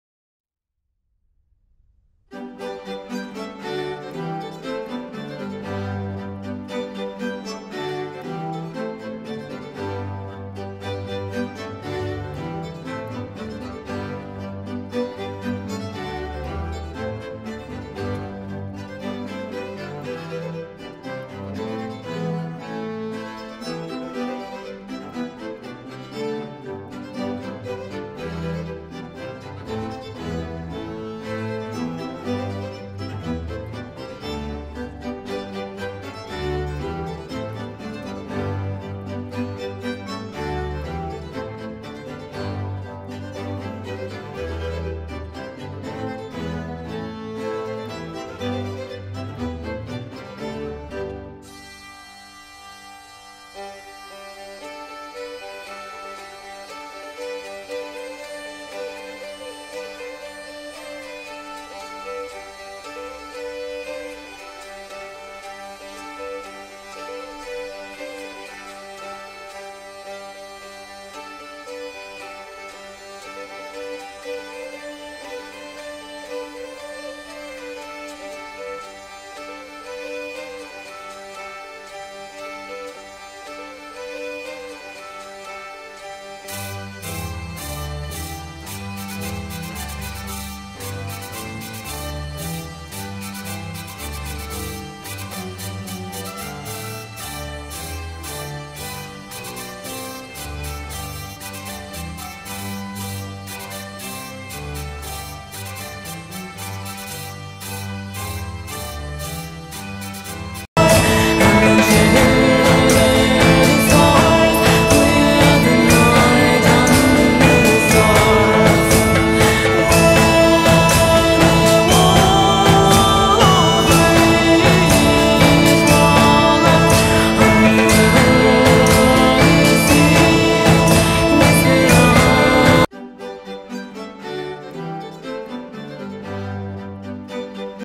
The Attack of the Castle
The king attacks the castle with a belfry and a catapult.